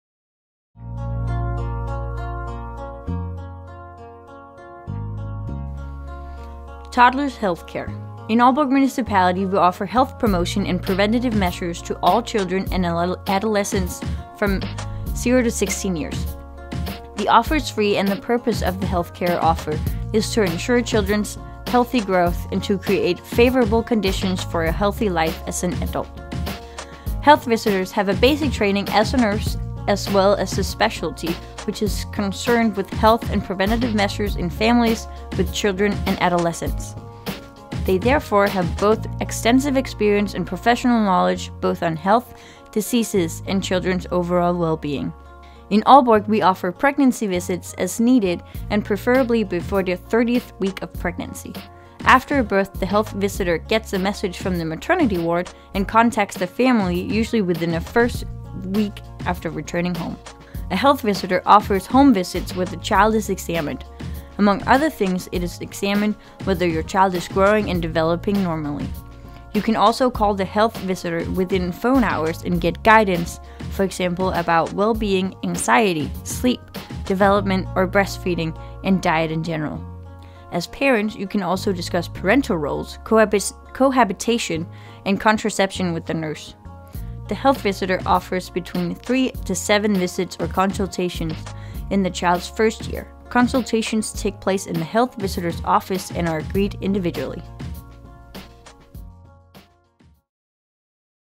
Introduction to the health-care for small children in Aalborg Kommune
Engelsksproget film, som introducerer til småbørnssundhedsplejens tilbud til forældre i Aalborg Kommune.